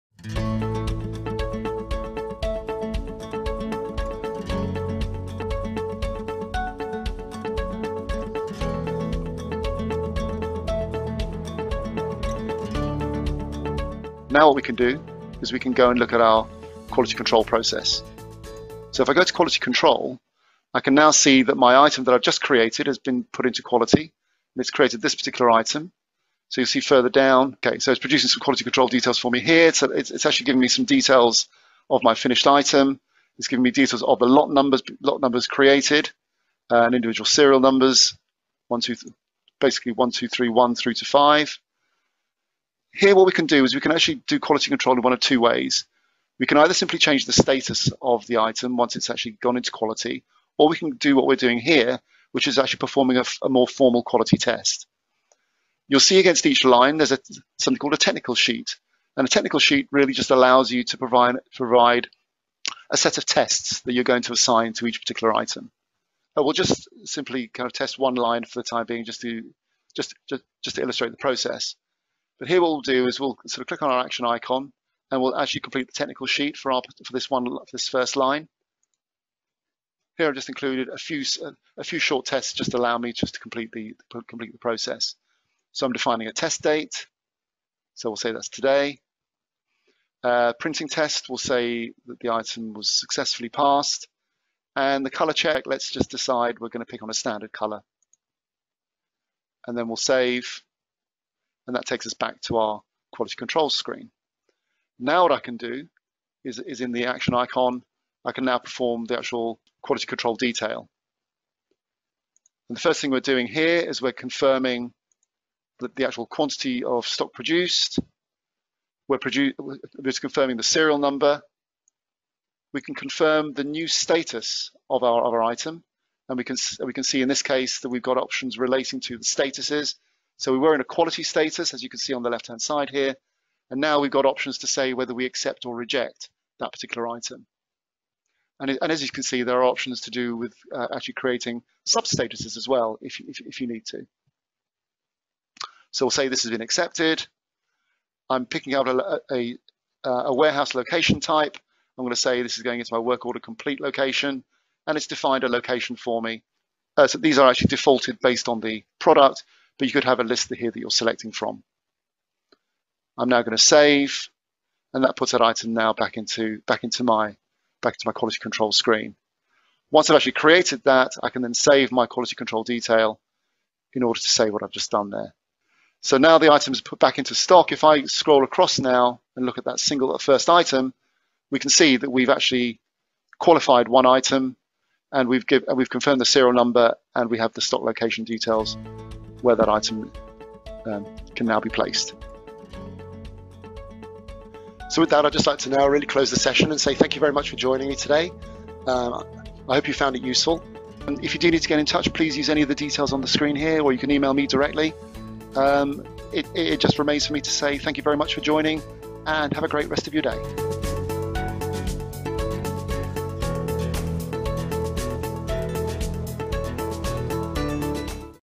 Sage X3 How To | The Quality Control Process
The quality control process occurs once production tracking has been completed in Sage X3. This episode discusses 1 of the 2 ways that you can carry out quality control in your solution.

We look at how to carry out a more formal quality test by completing technical sheets as well as confirming various details of an item. Once the item is approved via this process, it can go back into stock.